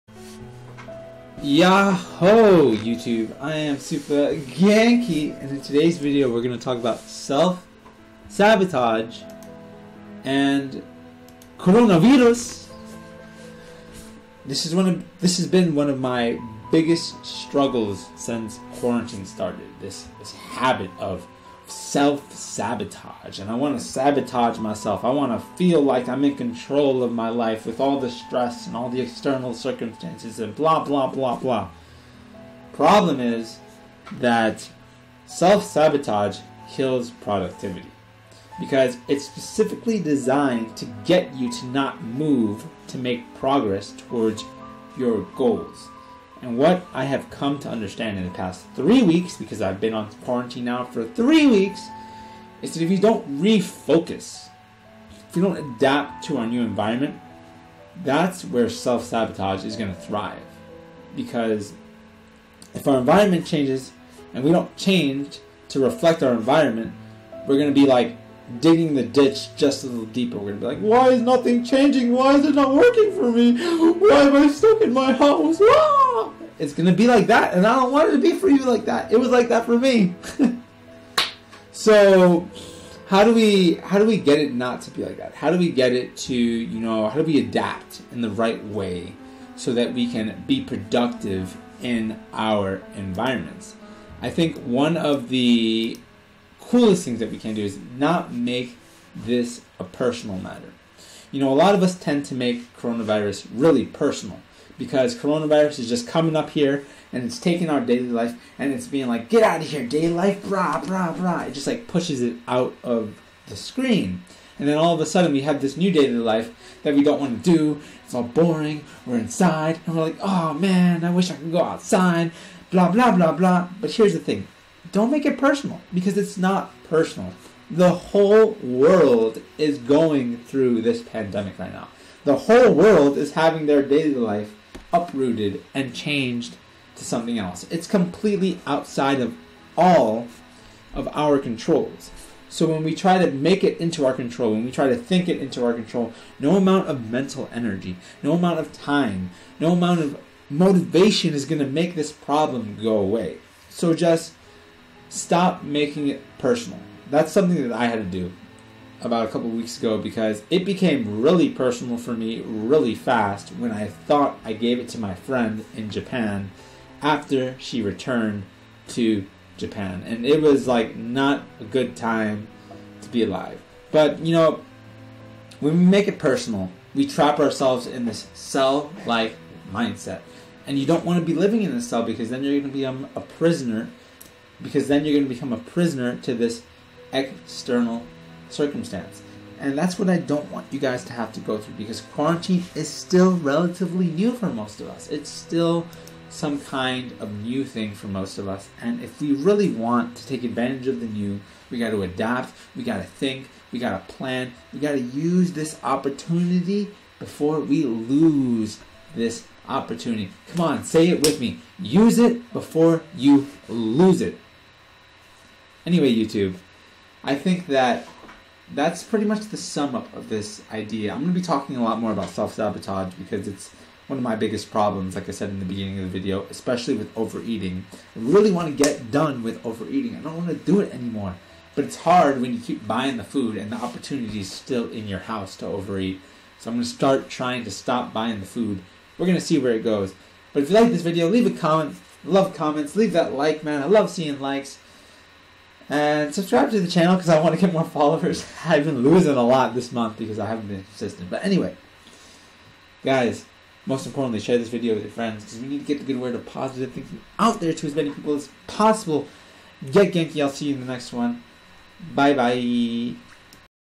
Self Sabotage and Quarantine
This is my biggest problem in quarantine and im sure its a problem for you to. In order to come to a better solution we need to stop making it so personal you know what im saying.

how? Watch the video to find out!